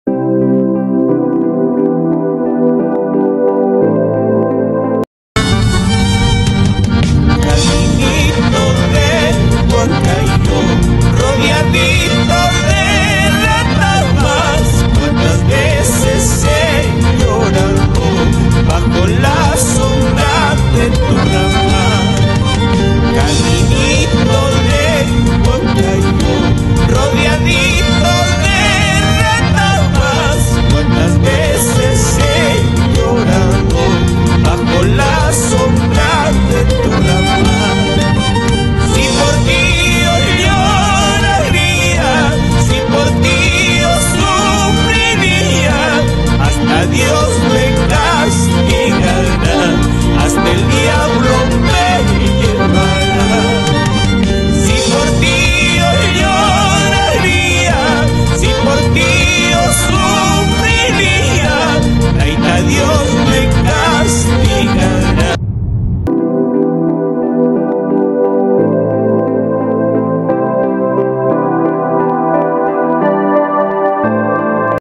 Visitando el Valle del Mantaro
Este video destaca una opinión reveladora con el Dr. Enrique Cornejo, un destacado economista, catedrático y exfuncionario del gobierno, quien ofrece una mirada crítica y constructiva sobre los desafíos multifacéticos que enfrenta Perú. Con su profundo conocimiento y experiencia, el Dr. Cornejo desgrana la crisis institucional y de valores en el país, aportando un análisis enriquecedor desde perspectivas política, técnica y académica

Enrique Cornejo